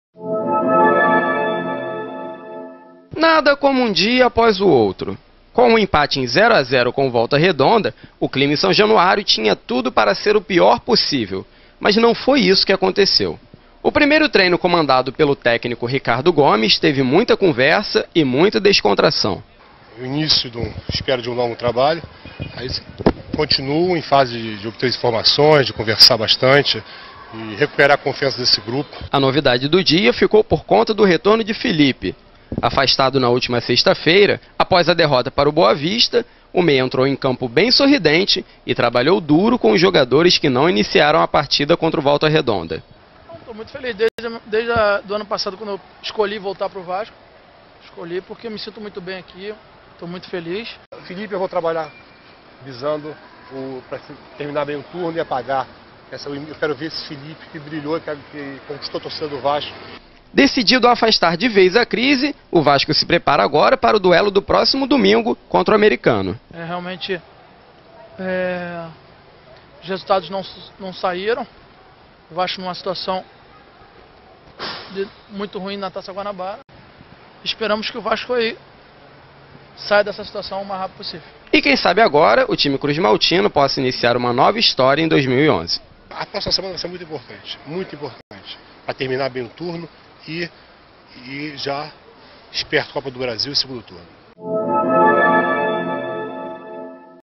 Ricardo Gomes comanda primeiro treino no Vasco
Em estreia de Ricardo Gomes e retorno de Felipe, Vasco faz treino descontraido em São Januário. Veja!